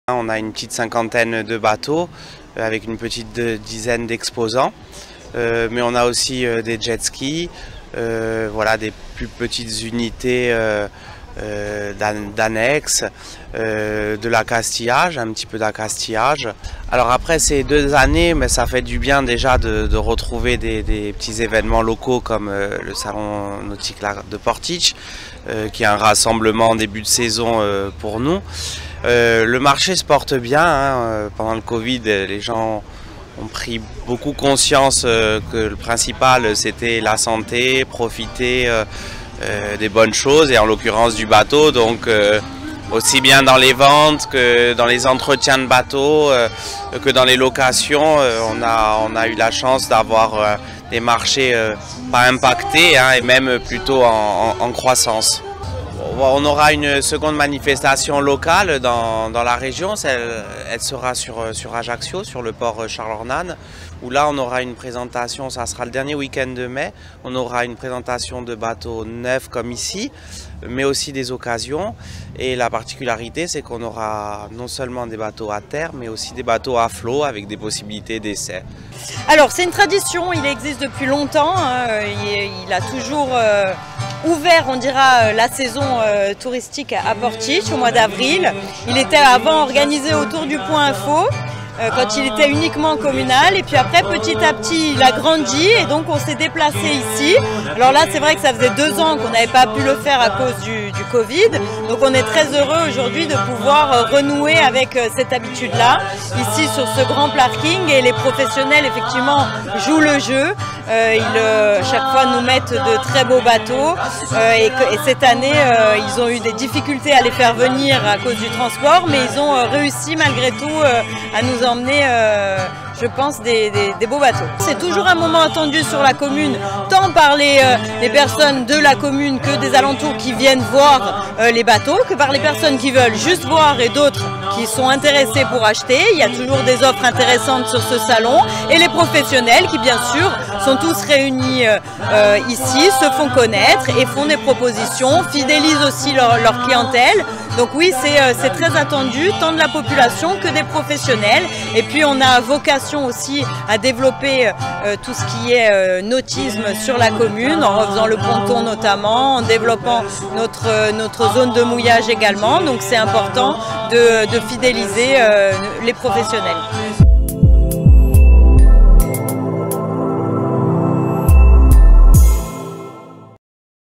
Retour du salon Nautique à Porticcio
Suspendu durant 2 ans suite à la pandémie,  le salon Nautic à fait son grand retour à Porticcio le weekend dernier .